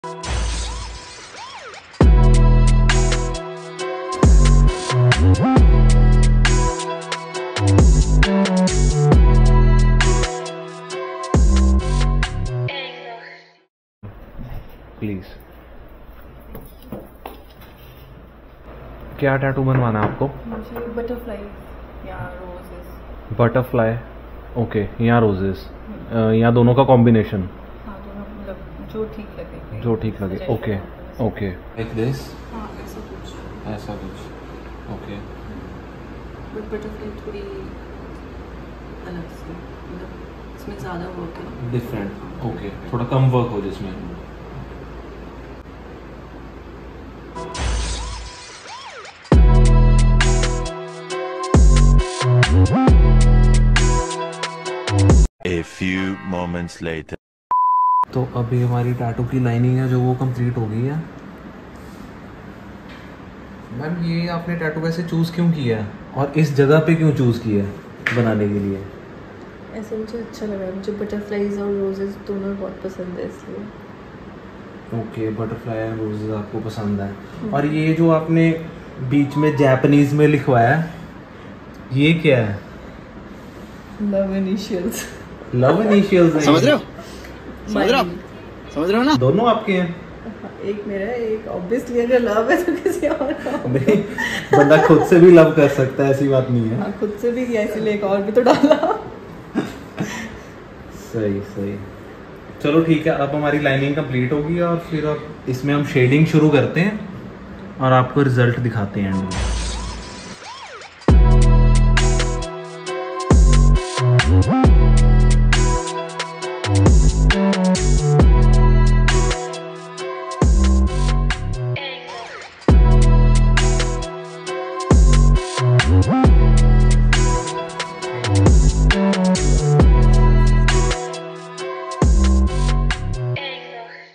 Vlog 2 - Making of a Beautiful BUTTERFLY TATTOO // Butterfly with Rose Tattoo // Marvels Ink Tattoos
Making of Butterfly Tattoo  at Marvels ink Tattoos,
we are situated in Chandigarh and Ludhiana